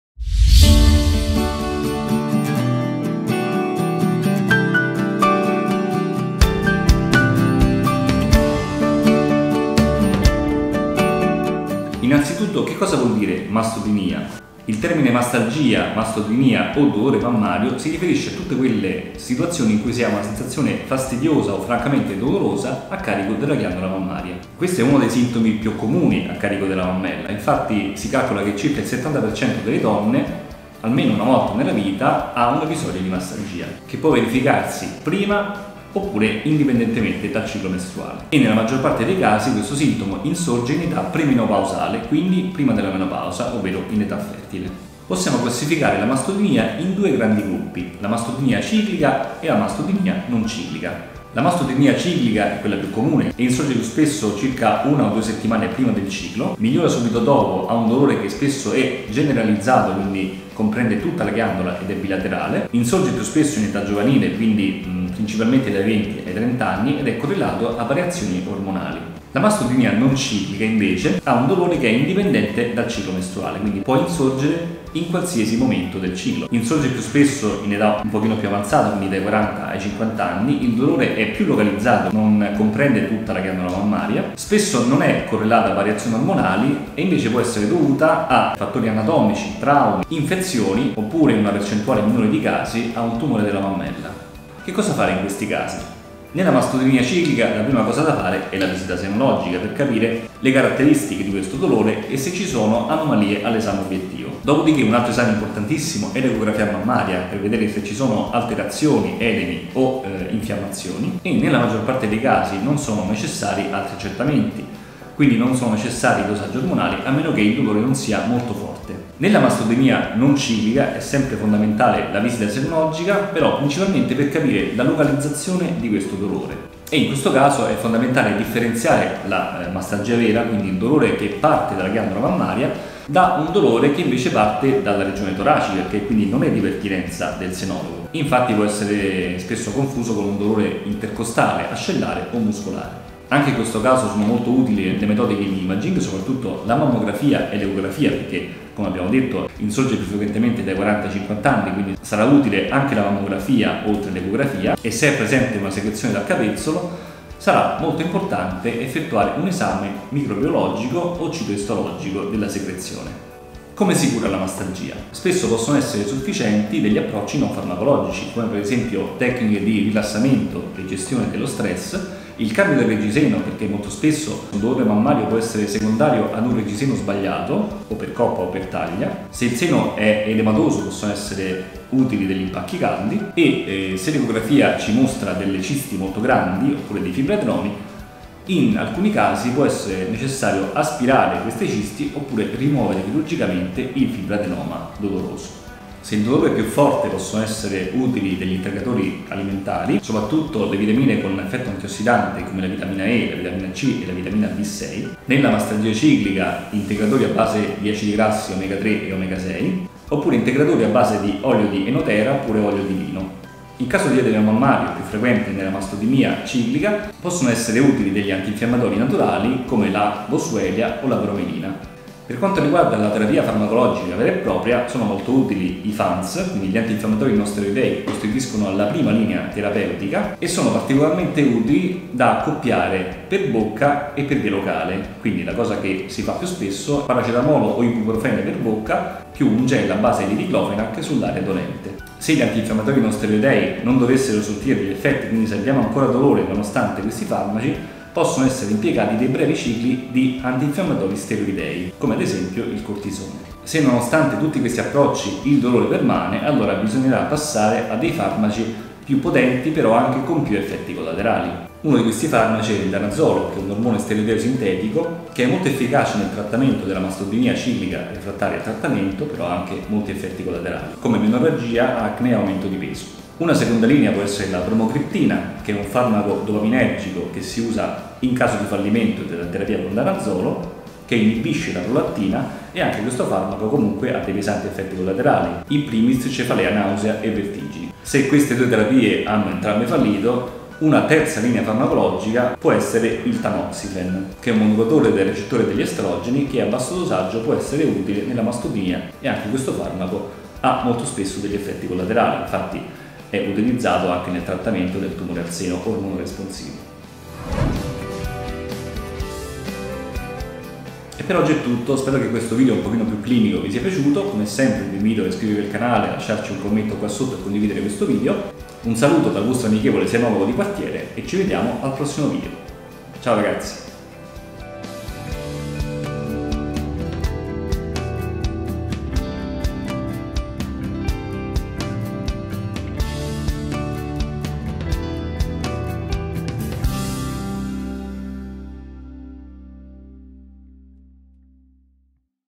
IL DOLORE AL SENO (O MASTODINIA) 👨‍⚕️ Il quaderno della paziente
📞Prenota una visita online cliccando quì o chiamando il numero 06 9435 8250

Ciao a tutti! In questo video, per la serie "il quaderno della paziente" il dott. Federico Frusone, chirurgo senologo, parlerà della mastodinia o dolore al seno. È una condizione frequentissima: circa il 70% delle donne almeno una volta nella vita ha accusato un dolore al seno, appena prima del ciclo (mastodinia ciclica) o indipendentemente dal ciclo mestruale (mastodinia non ciclica). Quindi non bisogna preoccuparsi, spesso è dovuto a delle "semplici" fluttuazioni ormonali e nella maggior parte dei casi il dolore può essere ridotto utilizzando integratori alimentari o blandi antinfiammatori.
Se avete altre domande scrivete quì sotto nei commenti!

I NOSTRI CANALI: